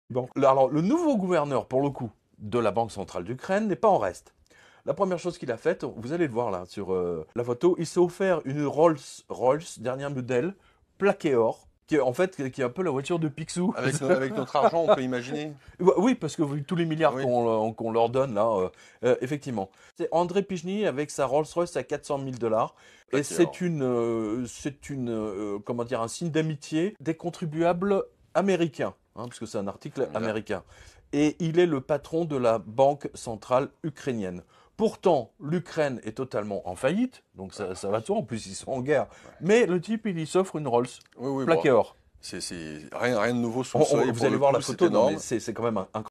Ukraine / une Rolls-Royce à 400 000$ (merci l'€urope)
le gouverneur de la bank centrale Ukrainienne se pays une roll plaquée or à  400 4000 $